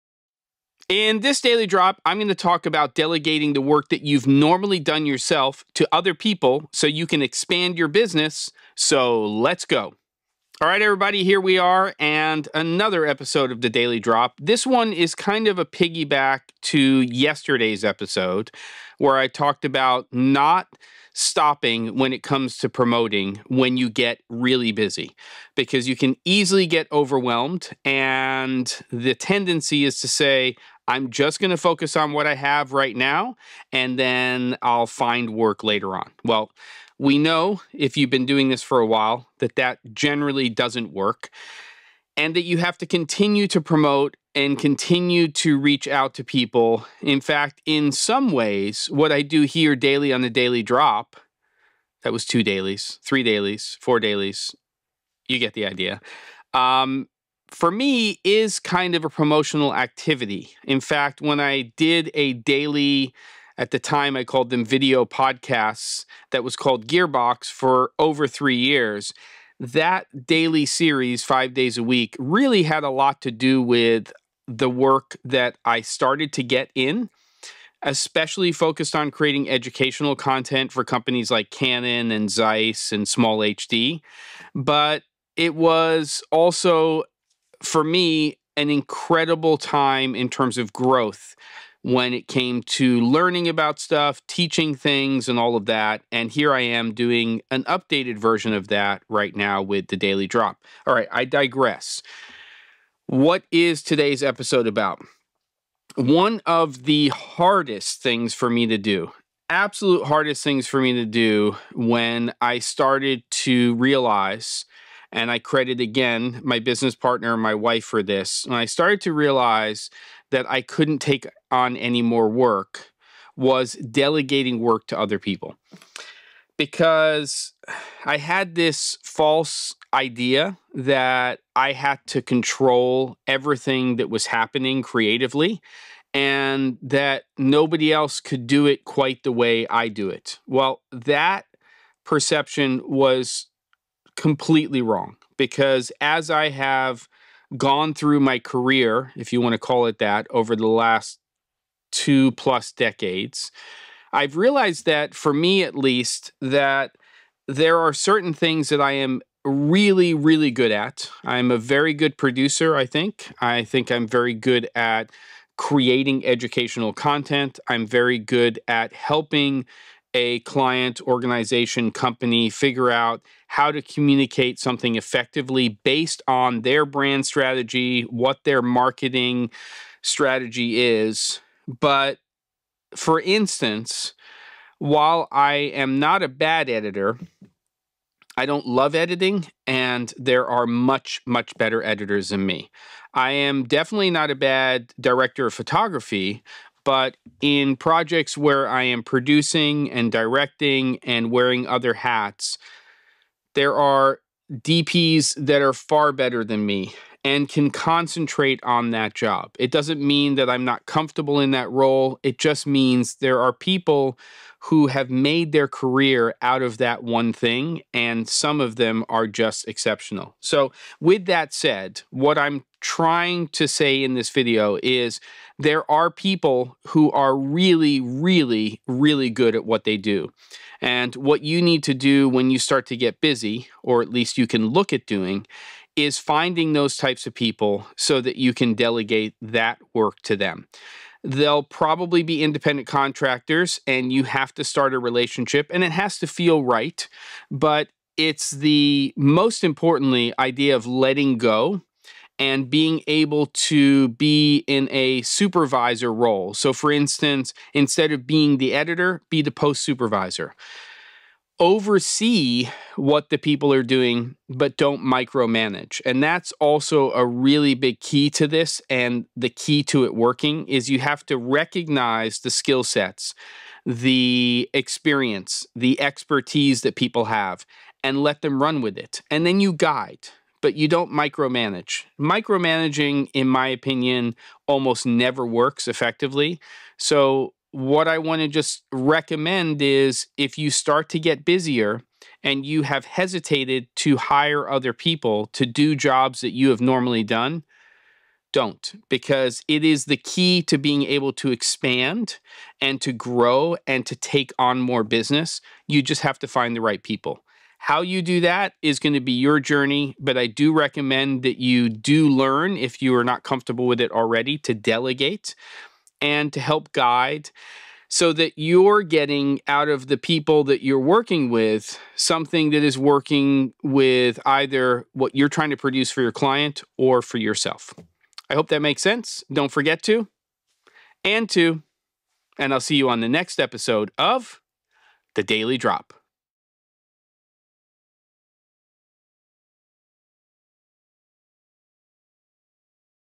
DELEGATING IS HARD, BUT ESSENTIAL! (EP.234)
As your business grows and you get busier, it's important to long term growth to recognize that you can't do it all alone. Learning to find and work with people who can do what you do as well, or better, than you is key expanding your business. It's not always easy to let go but it's essential to growth.

Products shown, mentioned and/or used in this episode...

Sony FX30

Sigma 18-50mm DC DN Lens

RØDE NTG5 Mic

Sound Devices MixPre-6 II